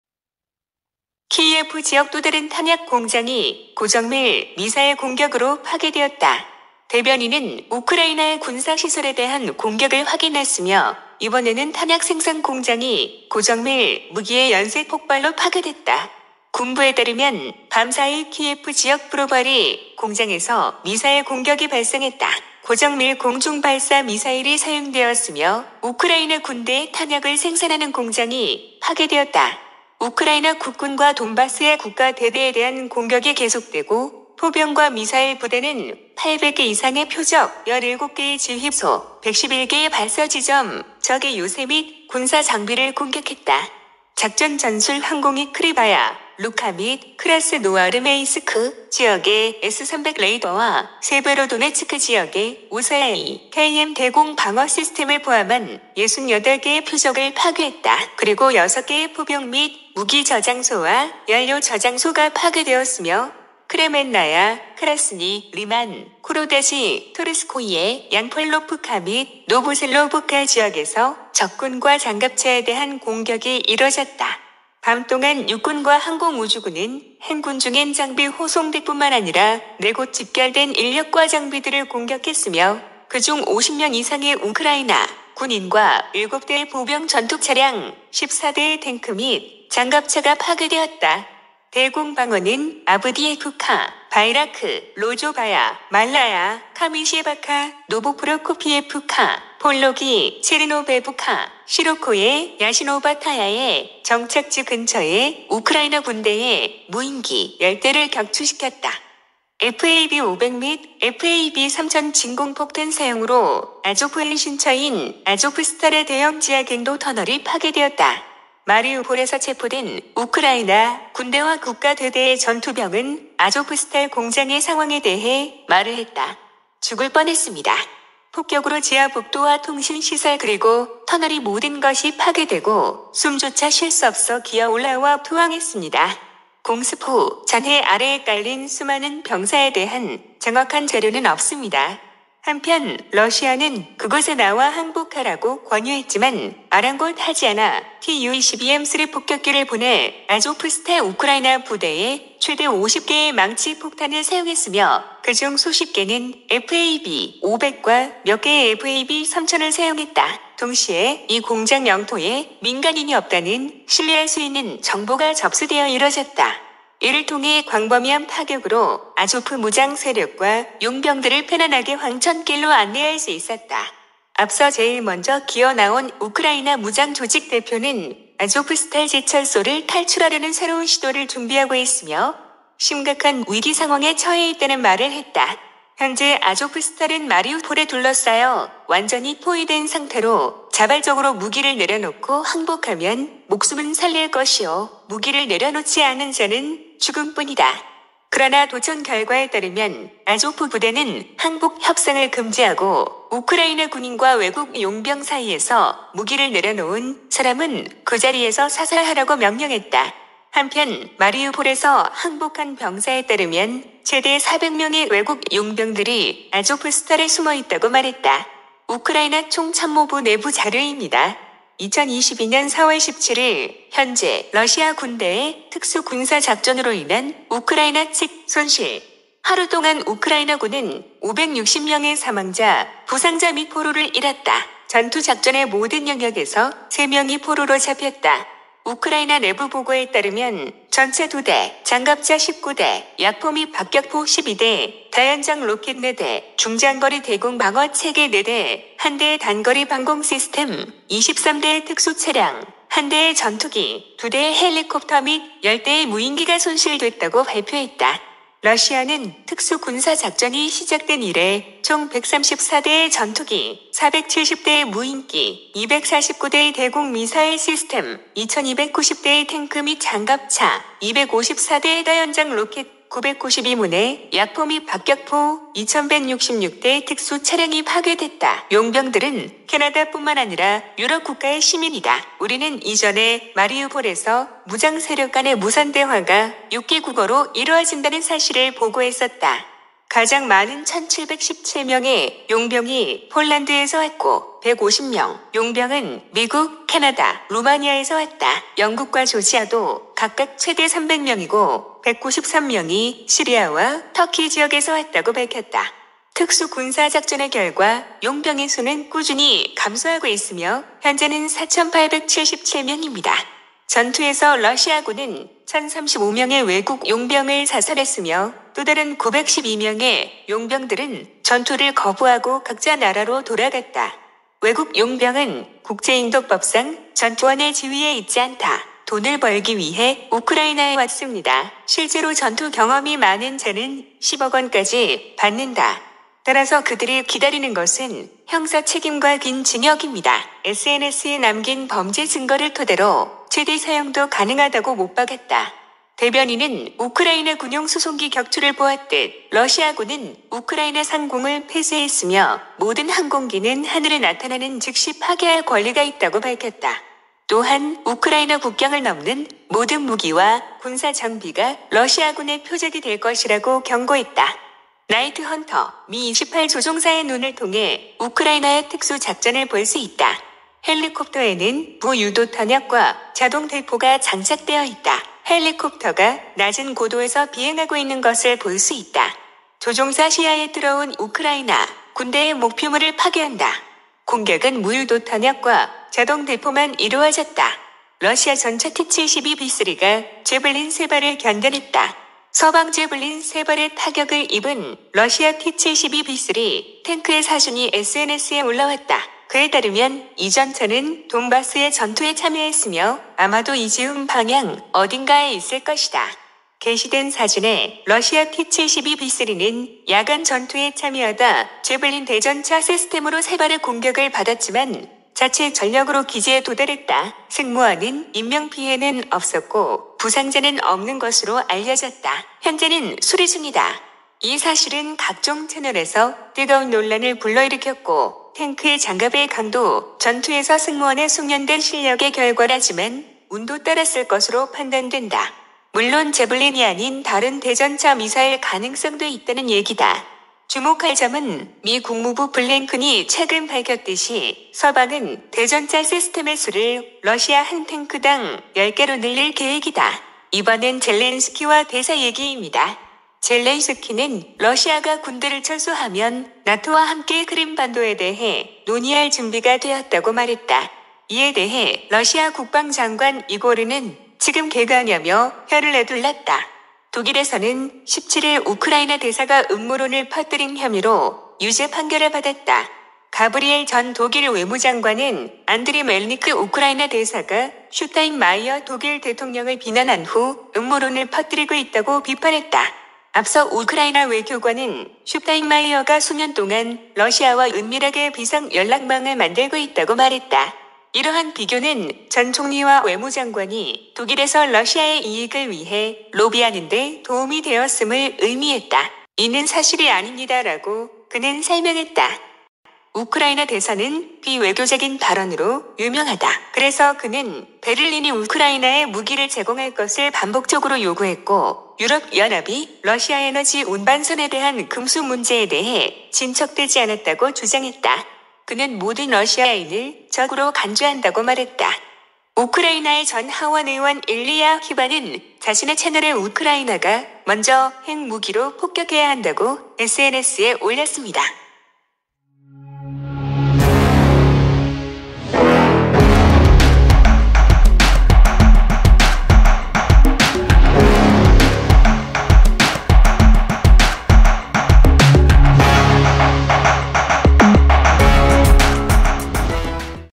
러시아 특수작전 17일 오전 브리핑 : 우크라이나 키예프 지역 또 다른 탄약 공장 강타!
키예프 지역 또 다른 탄약 공장이 고정밀 미사일 공격으로 파괴되었다.

대변인은 우크라이나의 군사 시설에 대한 공격을 확인했으며, 이번에는 탄약 생산 공장이 고정밀 무기에 연쇄 폭발로 파괴됐다.